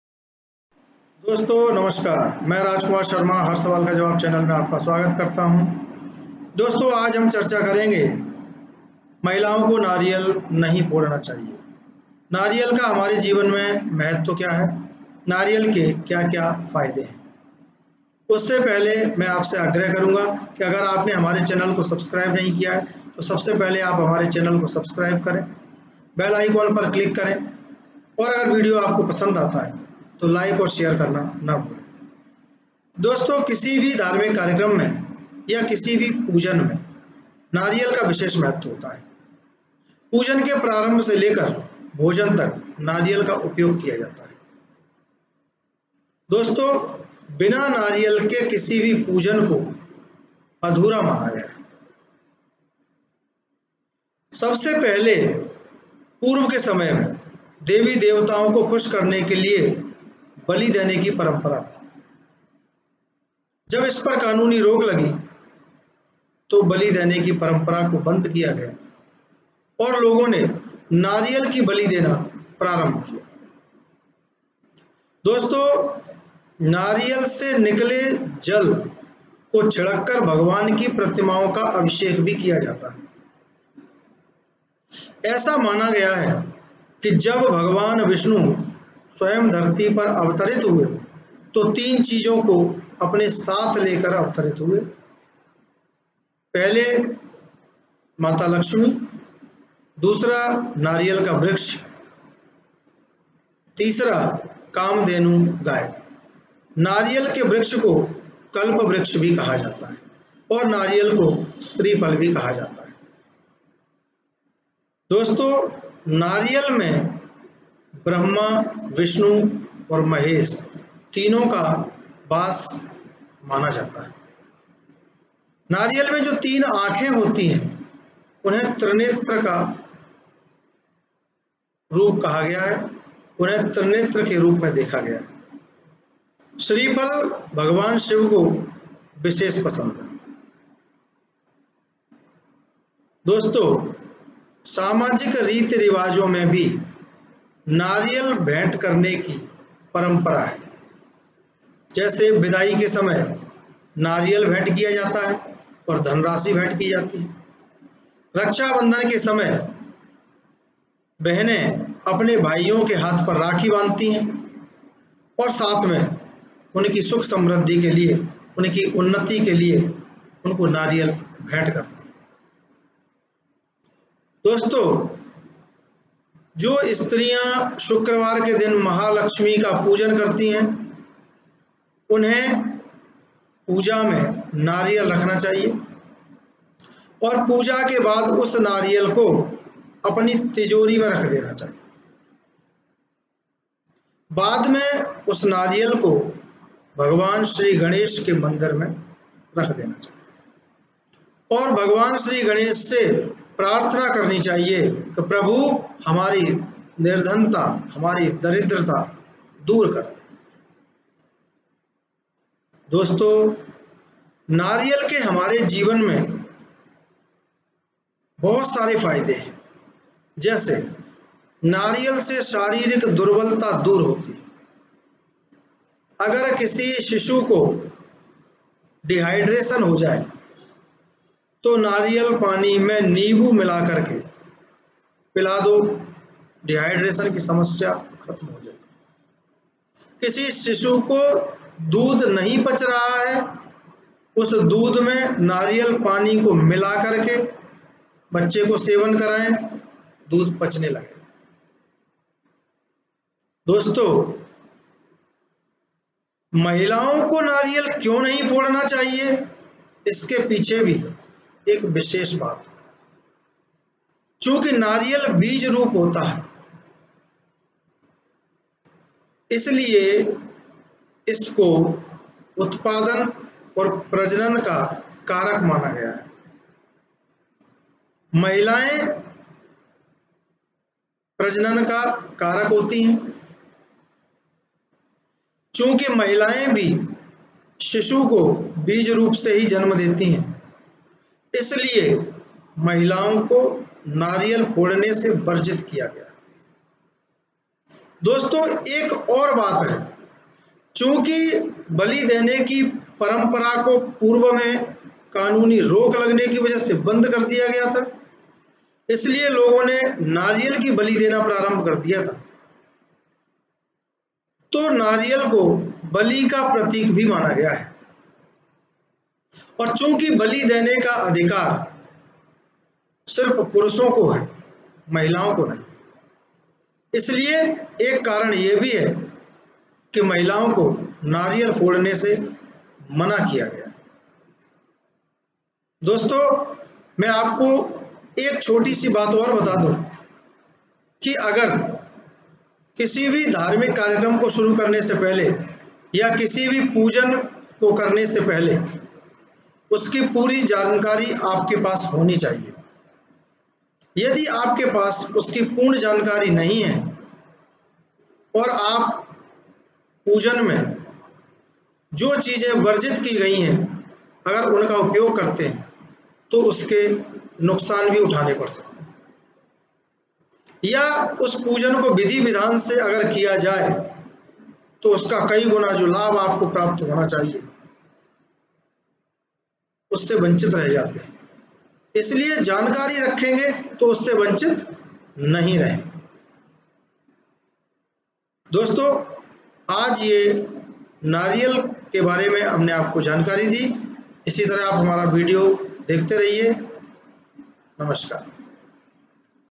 महिलाओं को नारियल कभी नहीं फोड़ना चाहिए, महिलाएँ क्यूँ नहीं फोड़ती नारियल, janiye nariyal ke fayde
Dosto namaskar is video mein yeh jankari di gayi hai ki mahilaon ko kabhi bhi nariyal nahi fodna chahiye. Aakhir is baat ke pichhe kya kya karan hain. Dusra ye ki nariyal ka hamare jivan me kya kya mahatva hai, avam hamare jivan mein nariyal ke kya kya fayde hain. Nariyal me Brahma, Vishnu aur Mahesh ka vaas hota hai. Bahut si aur bhi anokhi jankariyan pradan ki gayi hain jo aapke jivan ke liye bahut hi upyogi saabit hongi. full video dekhen aur subscribe karen like karen aur share bhi karen.